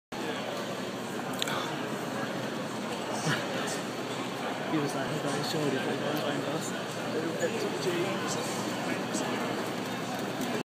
William Fichtner at LAX Airport 22.09.12
William Fichtner boarding flight V0024 from LAX, Los Angeles to London Heathrow